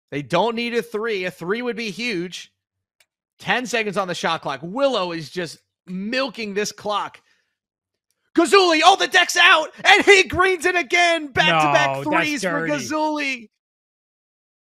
I CAN'T BELIEVE GAZULLI REALLY MADE $10K WITH THIS SHOT IN TRIPLE OT!!!🥶
or Each player's personal twitch stream

Player pro am game footage is uploaded with the written consent of the player.